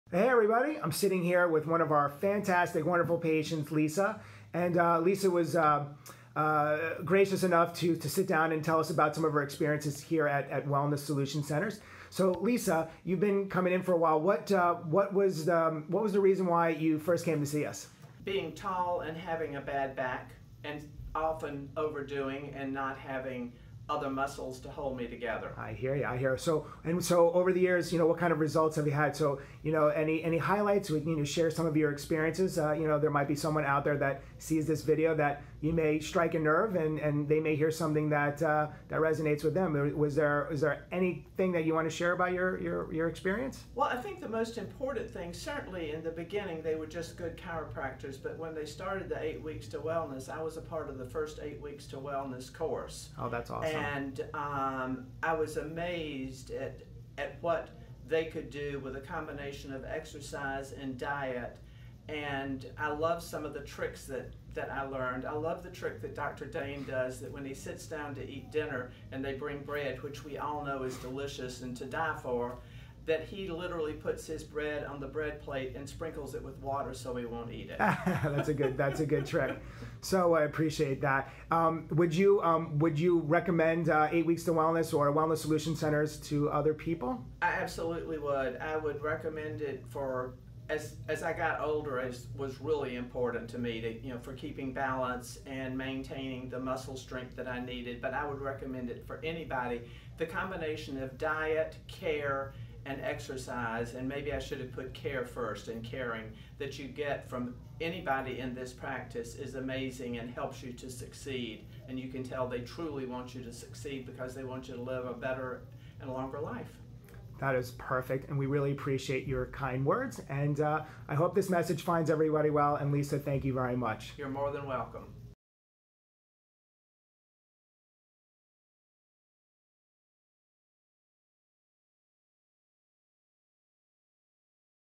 WSC - Lisa's Story - Newton PA
Know anyone skeptical of chiropractic? Send them in this video!

Lisa, one of our long-time patients, tells her story of coming to our practice, how chiropractic has helped, and her experience with 8 Weeks to Wellness!